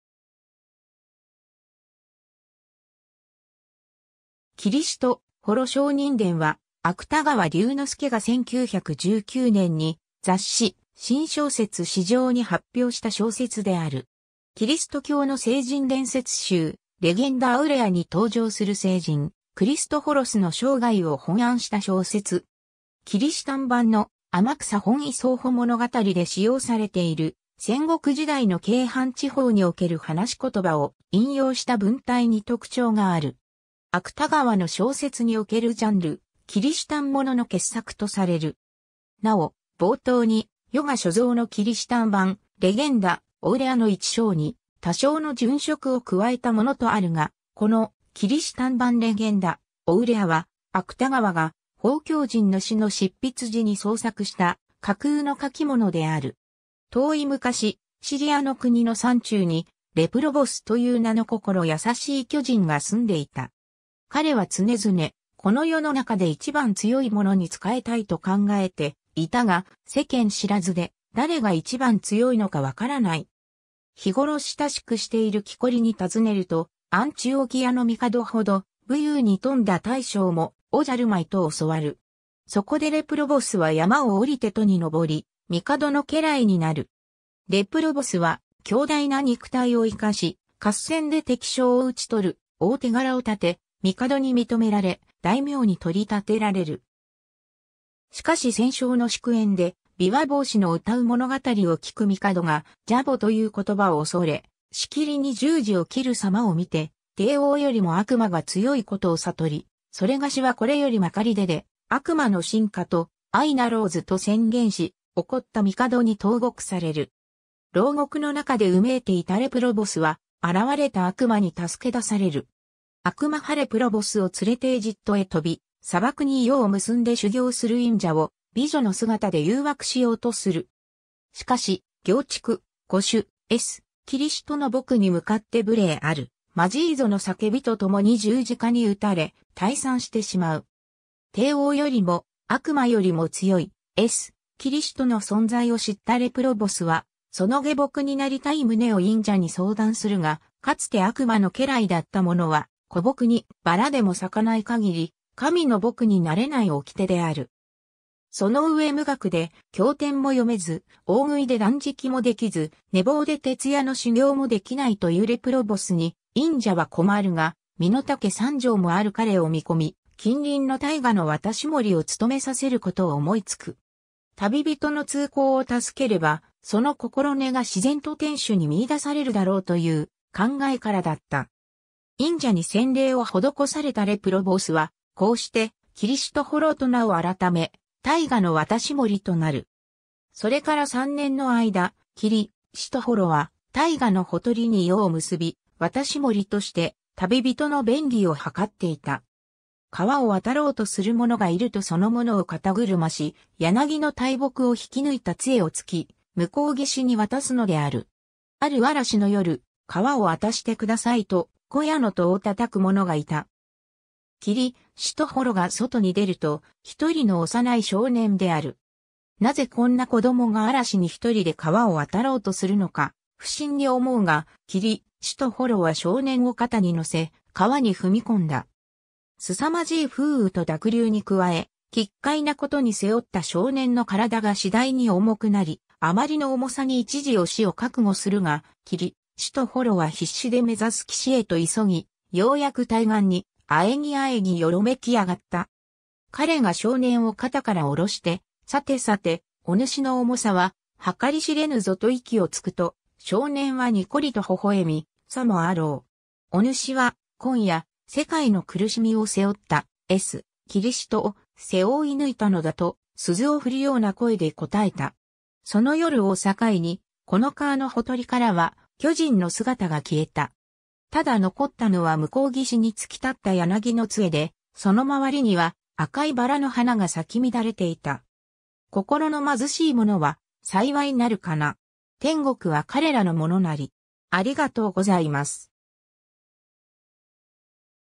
きりしとほろ上人伝
宗教・聖典を題材とした作品
『きりしとほろ上人伝』（きりしとほろしょうにんでん）は、芥川龍之介が1919年（大正8年）に雑誌『新小説』誌上に発表した小説である。
 キリスト教の聖人伝説集『レゲンダ・アウレア』（黄金伝説）に登場する聖人クリストフォロスの生涯を翻案した小説。
キリシタン版の『天草本伊曾保物語』（1594年刊）で使用されている、戦国時代の京阪地方における話し言葉を引用した文体に特徴がある。
芥川の小説におけるジャンル「切支丹物」の傑作とされる。
 なお、冒頭に「予が所蔵の切支丹版「れげんだ・おうれあ」の一章に、多少の潤色を加へたもの」とあるが、この『切支丹版れげんだ・おうれあ』は、芥川が『奉教人の死』の執筆時に創作した架空の書物である。
 遠い昔、「しりあ」の国の山中に、「れぷろぼす」という名の心優しい巨人が住んでいた。
彼は常々、この世の中で一番強いものに仕えたいと考えていたが、世間知らずで誰が一番強いのかわからない。
日ごろ親しくしている樵に尋ねると、「あんちおきやの帝ほど、武勇に富んだ大将もおじゃるまい」と教わる。
そこでれぷろぼすは山を下りて都に上り、帝の家来になる。
 れぷろぼすは強大な肉体を生かし、合戦で敵将を討ち取る大手柄をたて、帝に認められ、大名に取り立てられる。
しかし戦勝の祝宴で、琵琶法師の詠う物語を聞く帝が「じゃぼ」（悪魔）という言葉を恐れ、しきりに十字を切るさまを見て、帝王よりも悪魔が強いことを悟り、「それがしはこれよりまかり出で、悪魔の臣下と相成ろうず」と宣言し、怒った帝に投獄される。
牢獄の中で呻いていたれぷろぼすは、現れた悪魔に助け出される。
悪魔はれぷろぼすを連れてえじっとへ飛び、砂漠に庵を結んで修行する隠者を美女の姿で誘惑しようとする。
しかし「業畜、御主『えす・きりしと』の僕に向って無礼あるまじいぞ」の叫びと共に十字架に打たれ、退散してしまう。
 帝王よりも、悪魔よりも強い「えす・きりしと」の存在を知ったれぷろぼすは、その下僕になりたい旨を隠者に相談するが、かつて悪魔の家来だった者は、枯木にバラでも咲かない限り、神の僕になれない掟である。
そのうえ無学で教典も読めず、大食いで断食も出来ず、寝坊で徹夜の修行も出来ないと云うれぷろぼすに隠者は困るが、身の丈3丈もある彼を見込み、近隣の大河の渡し守を務めさせることを思いつく。
旅人の通行を助ければ、その心根が自然と天主に見出されるだろうという考えからだった。
隠者に洗礼を施されたれぷろぼすはこうして「きりしとほろ」と名を改め、大河の渡し守となる。
 それから三年の間、きりしとほろは大河のほとりに庵を結び、渡し守として旅人の便宜を図っていた。
河を渡ろうとする者がいるとその者を肩車し、柳の大木を引き抜いた杖をつき、向こう岸に渡すのである。
ある嵐の夜、「河を渡してください」と小屋の戸を叩く者がいた。
きりしとほろが外に出ると、一人の幼い少年である。
なぜこんな子供が嵐に一人で河を渡ろうとするのか不審に思うが、きりしとほろは少年を肩に乗せ、河に踏み込んだ。
 すさまじい風雨と濁流に加え、奇怪なことに背負った少年の体が次第に重くなり、あまりの重さに一時を死を覚悟するが、きりしとほろは必死で目ざす岸へと急ぎ、ようやく対岸に、喘ぎ喘ぎよろめき上がった。
彼が少年を肩から降ろして「さてさて、おぬしの重さは、はかり知れぬぞ」と息をつくと、少年はにこりと微笑み「さもあろう。おぬしは今夜、世界の苦しみを背負った『えす・きりしと』を、背負いぬいたのだ」と、鈴を振るような声で答えた。
 その夜を境に、この河のほとりからは巨人の姿が消えた。
ただ残ったのは向こう岸に突き立った柳の杖で、その周りには、赤いバラの花が咲き乱れていた。
 心の貧しい者は、幸いなるかな。
天国は彼らのものなり。